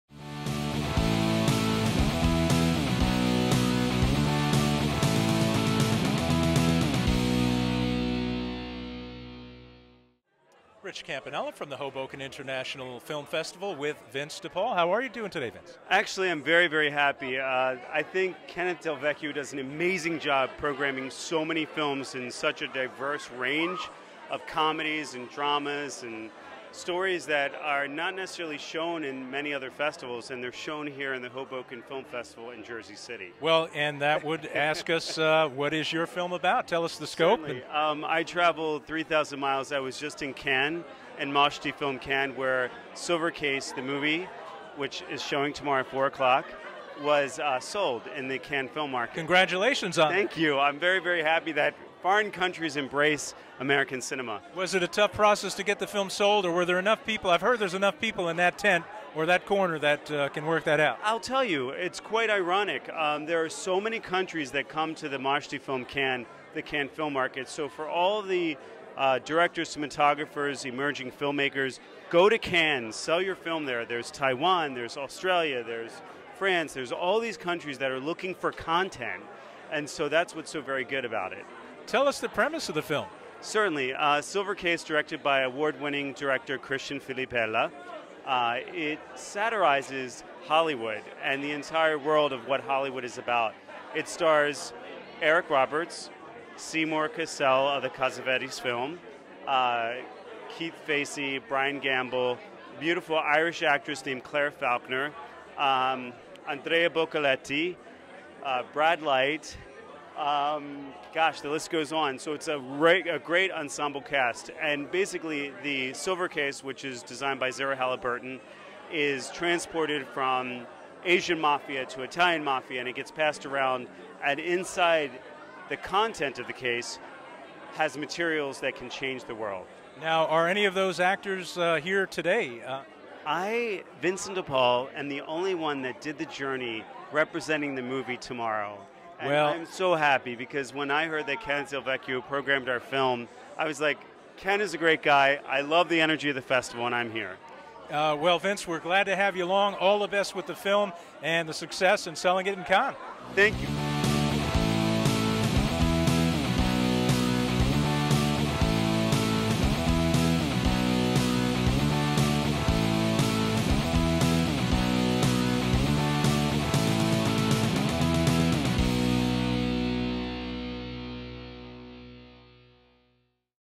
Interview with Vincent De Paul at Hoboken International Film Festival 2012! Red Carpet Event!
Interviews at the Hoboken international film festival 2012! red carpet event!

Interviewer: Rich Campanella

Camera: B.O.P. Team